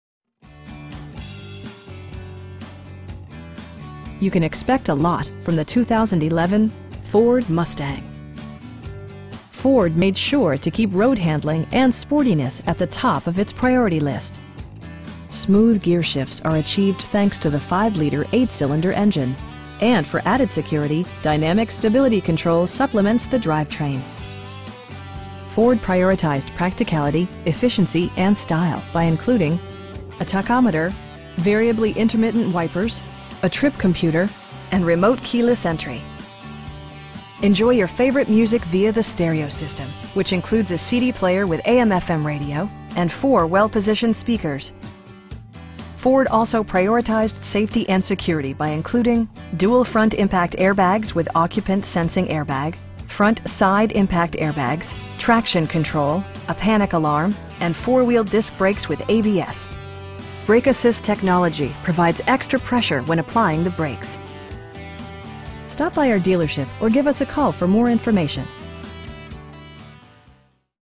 2011 Ford Mustang in San Juan Capistrano, CA 92675
You can expect a lot from the 2011 Ford Mustang. Ford made sure to keep road-handling and sportiness at the top of it's priority list. Smooth gearshifts are achieved thanks to the 5 liter 8 cylinder engine, and for added security, dynamic Stability Control supplements the drivetrain. Ford prioritized practicality, efficiency, and style by including: a tachometer, variably intermittent wipers, a trip computer, air conditioning, power door mirrors, cruise control, and remote keyless entry. Ford also prioritized safety and security by including: dual front impact airbags with occupant sensing airbag, front SIDE impact airbags, traction control, a panic alarm, and 4 wheel disc brakes with ABS. Brake assist technology provides extra pressure when applying the brakes. We know that you have high expectations, and we enjoy the challenge of meeting and exceeding them! Stop by our dealership or give us a call for more information.